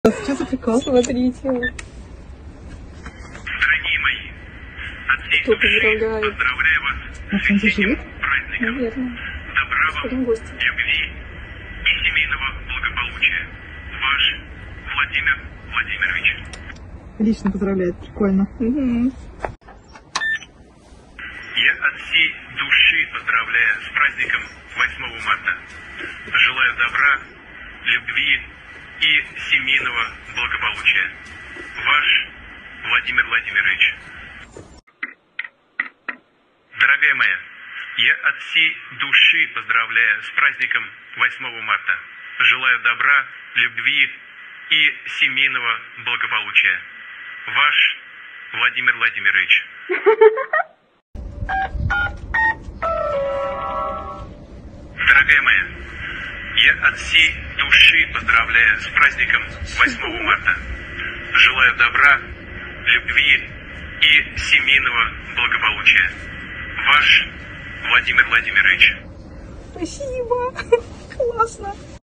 Умные домофоны поздравляют женщин с 8 Марта голосом Путина
Видео из разных городов публикуют в местных группах. Кто организовал акцию — неизвестно.